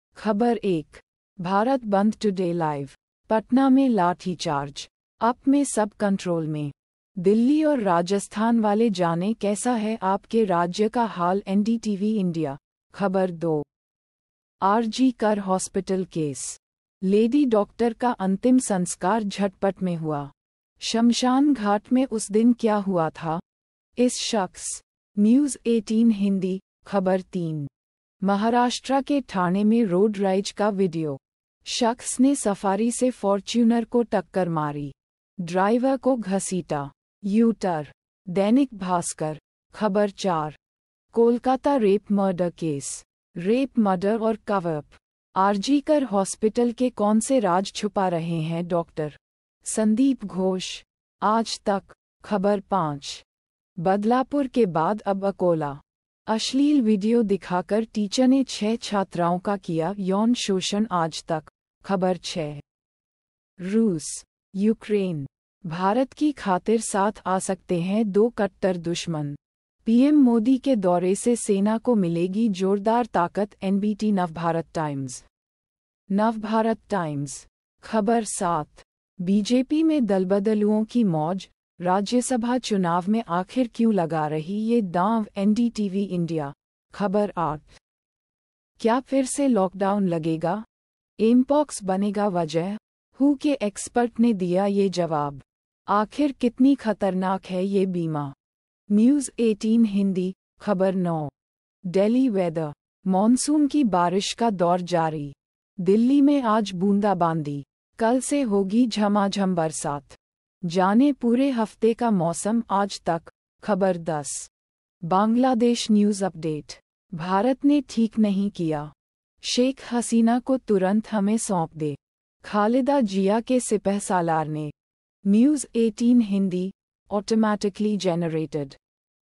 Hindi News (भारत)- 21/08/2024,04:43 PM GMT+5:30
ख़बर 1:Bharat Bandh Today LIVE: पटना में लाठीचार्ज, UP में सब कंट्रोल में, दिल्ली और राजस्थान वाले जानें- कैसा है आपके राज्य का हाल - NDTV India
ख़बर 2:RG Kar Hospital Case: लेडी डॉक्टर का अंतिम संस्कार झटपट में हुआ, श्मशान घाट में उस दिन क्या हुआ था? इस शख्स... - News18 हिंदी
ख़बर 3:महाराष्ट्र के ठाणे में रोड रेज का VIDEO: शख्स ने सफारी से फॉर्च्यूनर को टक्कर मारी, ड्राइवर को घसीटा; यू-टर... - Dainik Bhaskar
ख़बर 4:Kolkata Rape Murder Case: रेप-मर्डर और कवरअप... आरजी कर हॉस्पिटल के कौन से राज छुपा रहे हैं डॉ. संदीप घोष? - Aaj Tak
ख़बर 5:बदलापुर के बाद अब अकोला, अश्लील वीडियो दिखाकर टीचर ने 6 छात्राओं का किया यौन शोषण - Aaj Tak
ख़बर 6:रूस, यूक्रेन... भारत की खातिर साथ आ सकते हैं दो कट्टर दुश्‍मन, पीएम मोदी के दौरे से सेना को मिलेगी जोरदार ताकत - NBT नवभारत टाइम्स (Navbharat Times)
ख़बर 7:बीजेपी में दलबदलुओं की मौज,राज्यसभा चुनाव में आखिर क्यों लगा रही ये दांव - NDTV India
ख़बर 8:क्या फिर से लॉकडाउन लगेगा? एमपॉक्स बनेगा वजह? WHO के एक्सपर्ट ने दिया ये जवाब, आखिर कितनी खतरनाक है यह बीमा... - News18 हिंदी
ख़बर 9:Delhi Weather: मॉनसून की बारिश का दौर जारी, दिल्ली में आज बूंदाबांदी, कल से होगी झमाझम बरसात, जानें पूरे हफ्ते का मौसम - Aaj Tak
ख़बर 10:Bangladesh News Update: भारत ने ठीक नहीं किया, शेख हसीना को तुरंत हमें सौंप दे, खालिदा जिया के सिपहसालार ने... - News18 हिंदी

Automatically Generated...